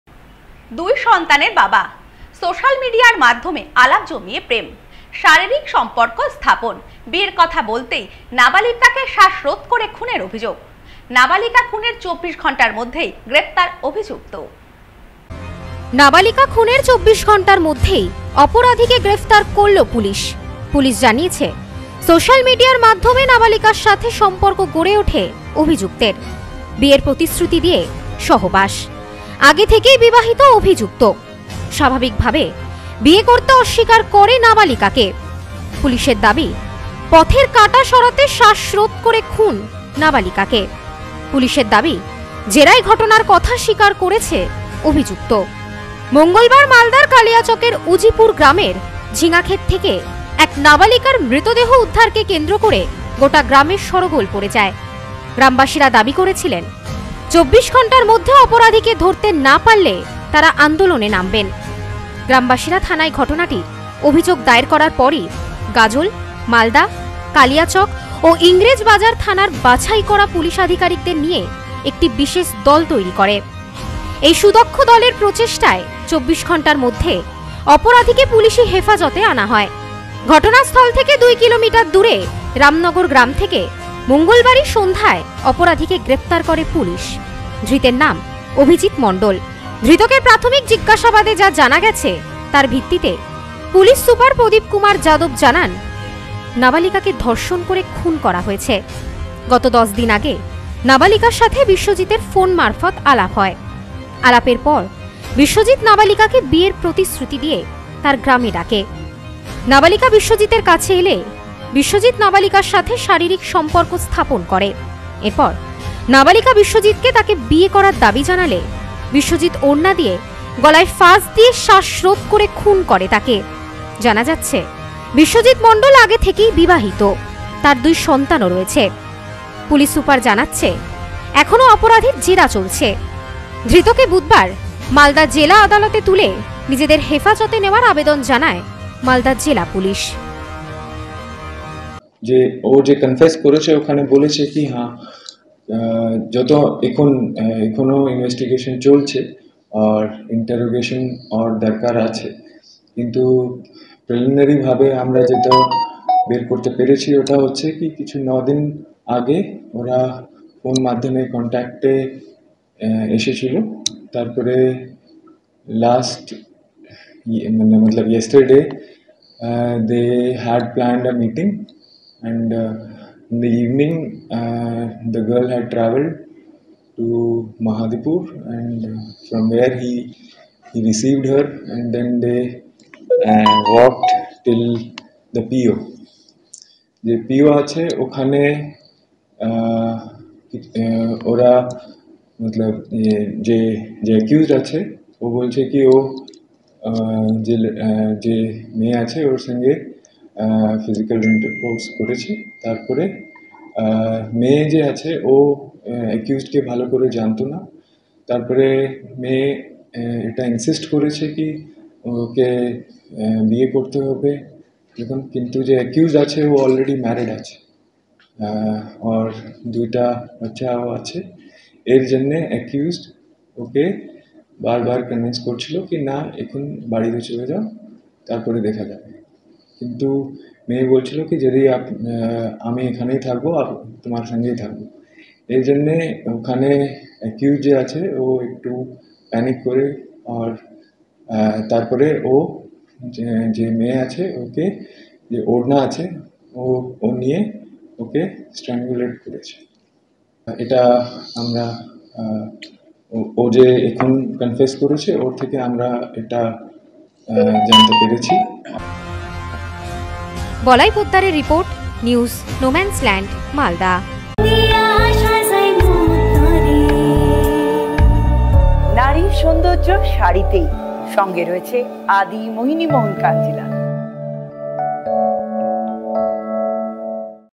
দুই সন্তানের পিতা, সোশ্যাল মিডিয়ার মাধ্যমে আলাপ জমিয়ে প্রেম। শারীরিক সম্পর্ক স্থাপন।
দুই সন্তানের পিতা, সোশ্যাল মিডিয়ার মাধ্যমে আলাপ জমিয়ে প্রেম। শারীরিক সম্পর্ক স্থাপন। বিয়ের কথা বলতেই শ্বাসরোধ করে খুনের অভিযোগ। নাবালিকা খুনের ২৪ ঘন্টার মধ্যে গ্রেপ্তার অভিযুক্ত।